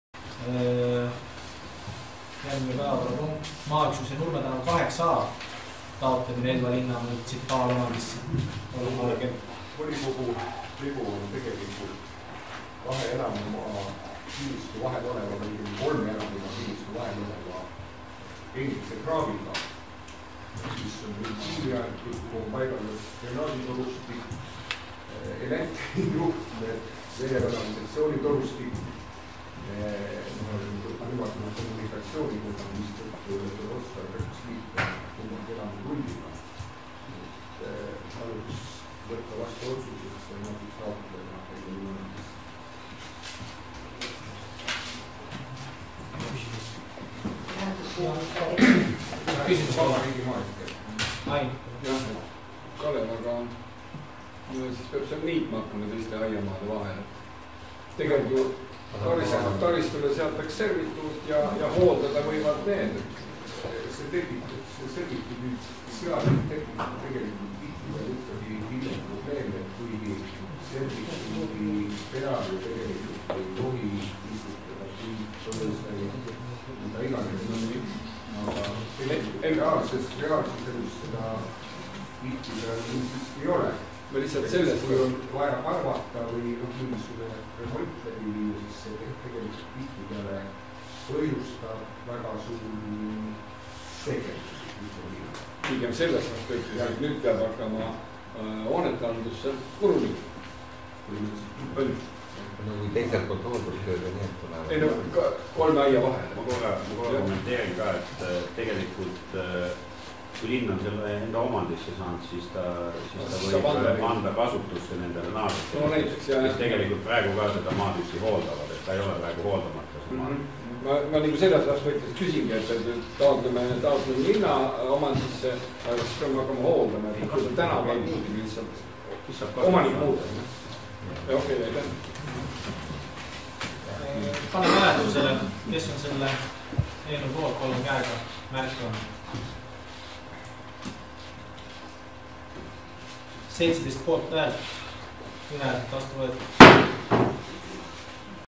Maaüksuse Nurme tn 8a taotlemine Elva linna munitsipaalomandisse - 18.11.2013
Maaüksuse Nurme tn 8a taotlemine Elva linna munitsipaalomandisse
Ettekandja: Kalev Kepp